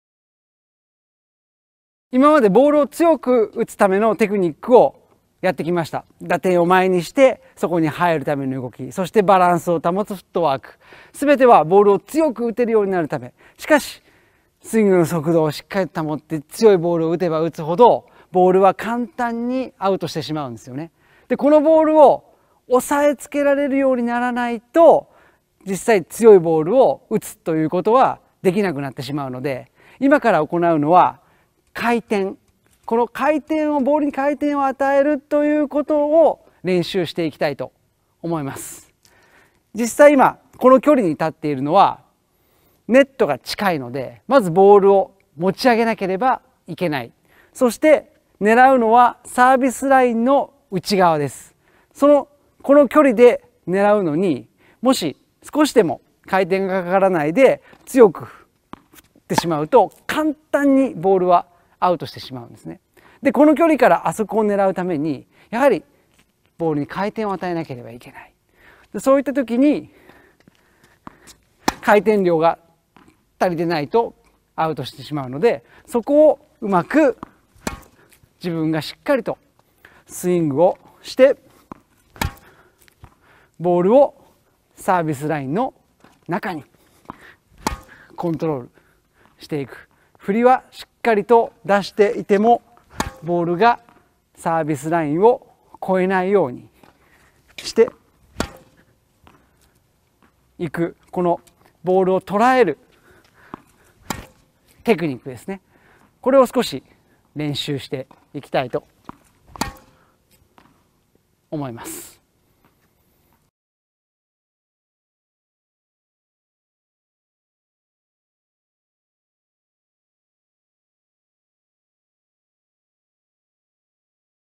１５強くボールを打つこととセットで覚えること_増田健太郎プロDVD
「どうしたらプロテニス選手のように思い切り打てるのか？」_増田健太郎プロDVD